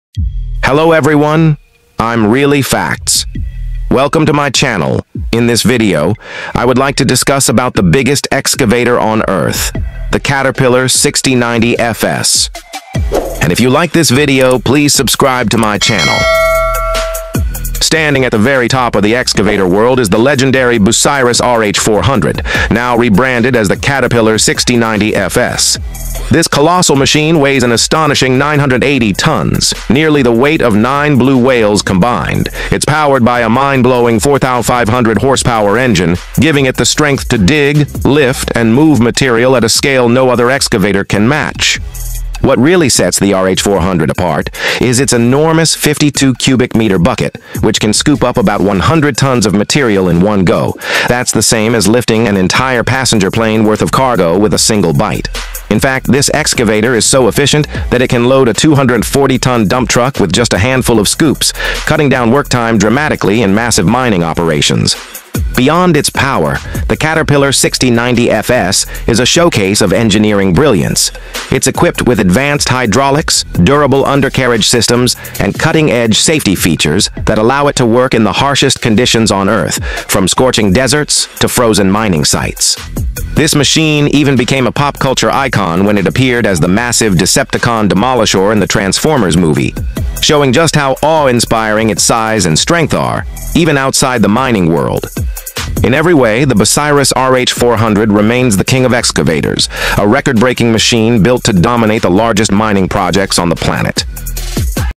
Caterpillar 6090 FS the "Monster Machine" that can Move Mountains!😲
Caterpillar 6090 FS

Standing at the very top of the excavator world is the legendary Bucyrus RH400, now rebranded as the Caterpillar 6090 FS. This colossal machine weighs an astonishing 980 tons, nearly the weight of nine blue whales combined. It’s powered by a mind-blowing 4,500 horsepower engine, giving it the strength to dig, lift, and move material at a scale no other excavator can match.

What really sets the RH400 apart is its enormous 52-cubic-meter bucket, which can scoop up about 100 tons of material in one go—that’s the same as lifting an entire passenger plane worth of cargo with a single bite. In fact, this excavator is so efficient that it can load a 240-ton dump truck with just a handful of scoops, cutting down work time dramatically in massive mining operations.

Beyond its power, the Caterpillar 6090 FS is a showcase of engineering brilliance. It’s equipped with advanced hydraulics, durable undercarriage systems, and cutting-edge safety features that allow it to work in the harshest conditions on Earth, from scorching deserts to frozen mining sites.

This machine even became a pop culture icon when it appeared as the massive Decepticon Demolishor in the Transformers movie, showing just how awe-inspiring its size and strength are—even outside the mining world.

In every way, the Bucyrus RH400 remains the king of excavators, a record-breaking machine built to dominate the largest mining projects on the planet.